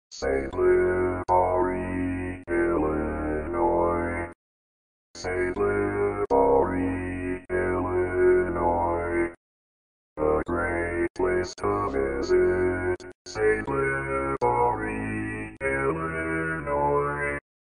How to Say or Pronounce USA Cities — Saint Libory, Illinois
This video shows you how to say or pronounce Saint Libory, Illinois.

A computer said Saint Libory, Illinois. How would you say Saint Libory, Illinois?